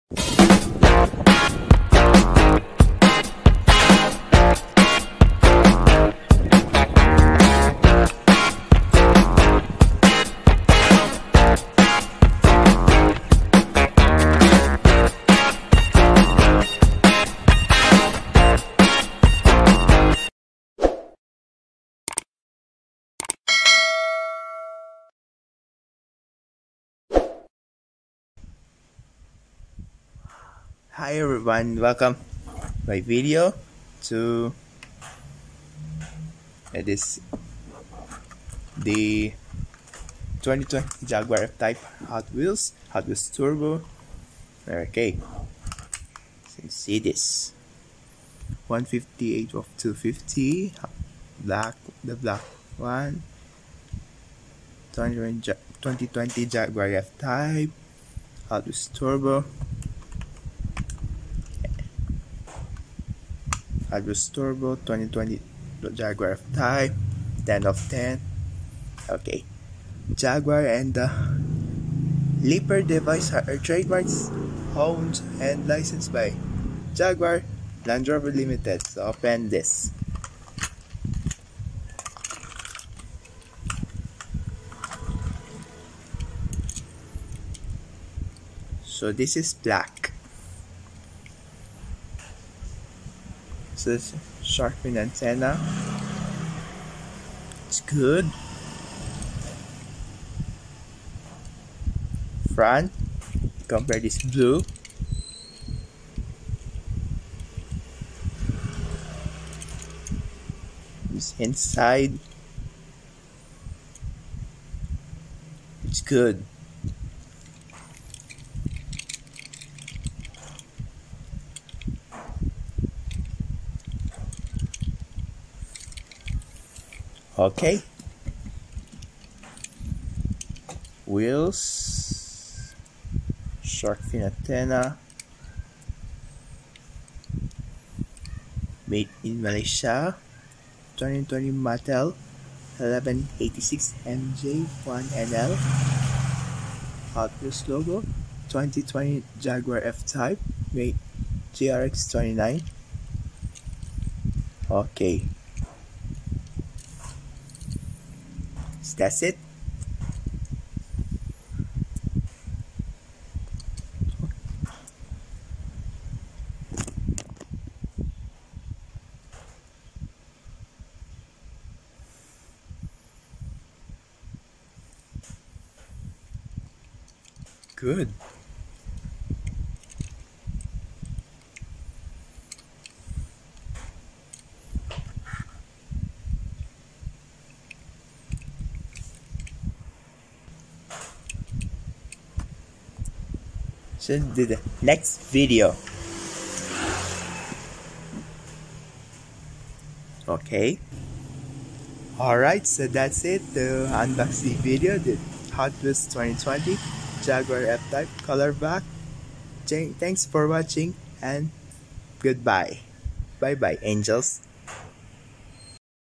Unboxed: HOT WHEELS 2020 JAGUAR F-TYPE COLOR BLACK
Hi Everyone. Welcome to my Video This Hot Wheels 2020 Jaguar F-Type Color Black

2022 Hot Wheels
HW Turbo 10/10
Hot Wheels 158/250
2020 Jaguar F-Type Color Black